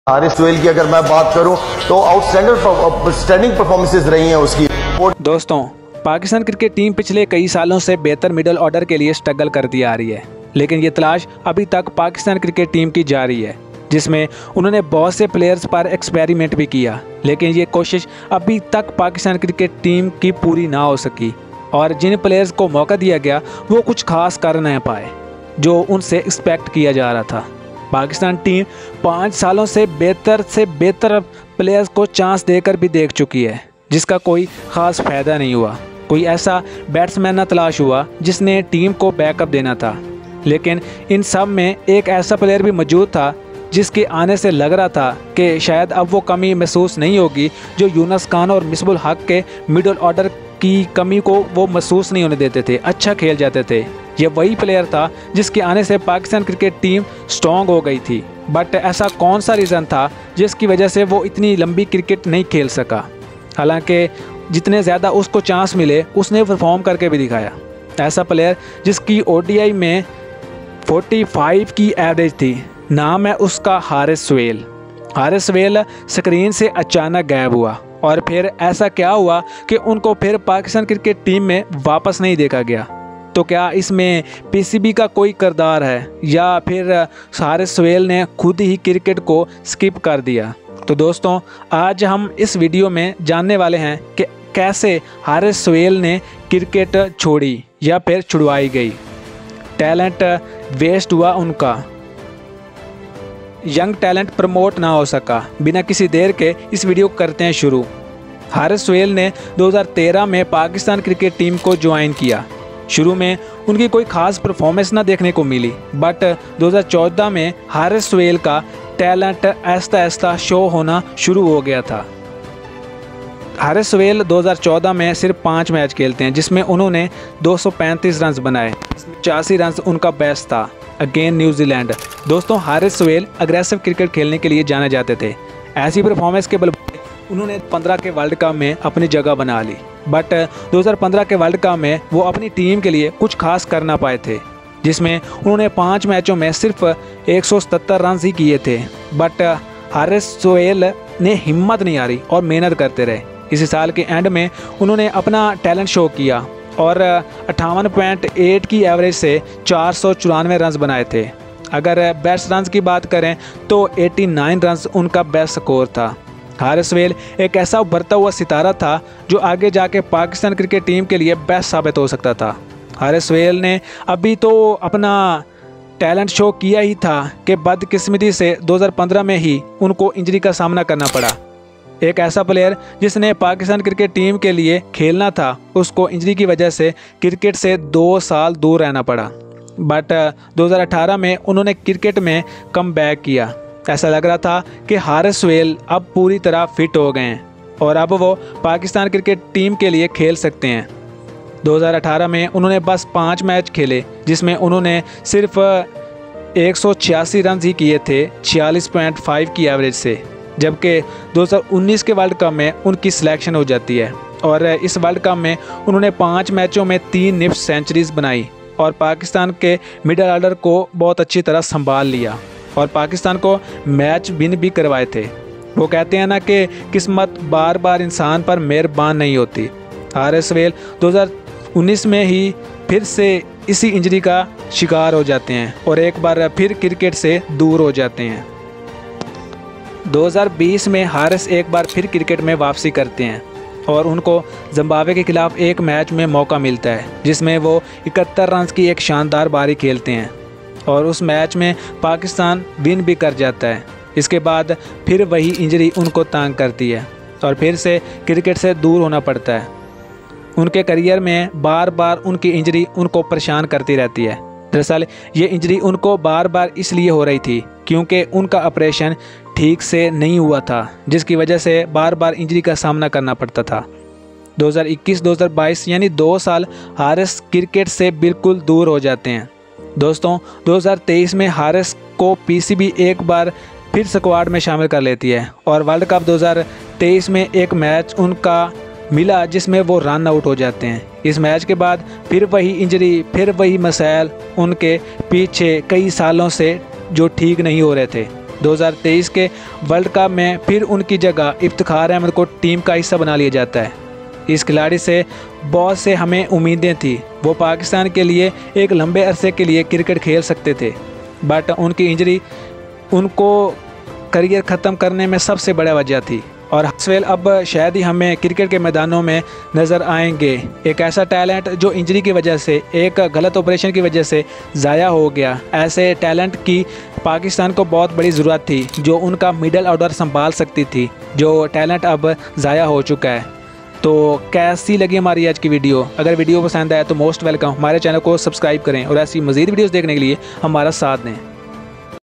Haris Sohail: The Journey of Pakistan's Left-Handed Maestro
Join us as we explore the remarkable cricketing journey of Harish Sohail, Pakistan's talented left-handed batsman. From his early days in domestic cricket to his shining moments on the international stage, we'll delve into his career highlights, challenges, and contributions to Pakistan cricket. Witness the story of determination, skill, and passion that defines Harish Sohail's legacy